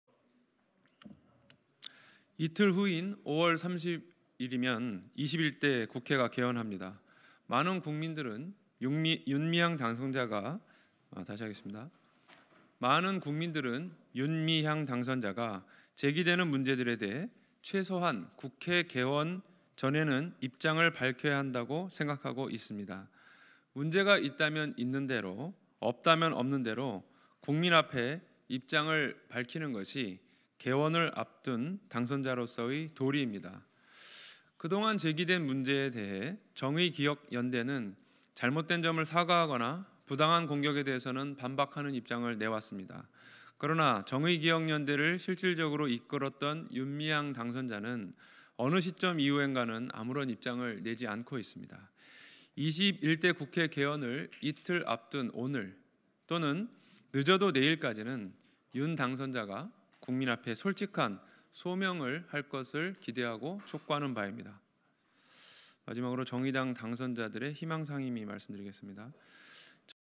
정의당 "윤미향은 서둘러 입장 내놔라! 이틀 밖에 안 남았다. 오늘 아니면 내일 중으로 꼭 소명과 입장을 내놔야 한다!“
[박귀성 기자]=정의당 김종철 대변인이 28일 오전 국회 소통관에서 논평을 내고 최근 논란이 거듭되고 있는 더불어민주당 윤미향 당선인에 대해 의혹이 계속 불거지는 만큼 소명과 입장 표명이 있어야 한다고 독촉했다.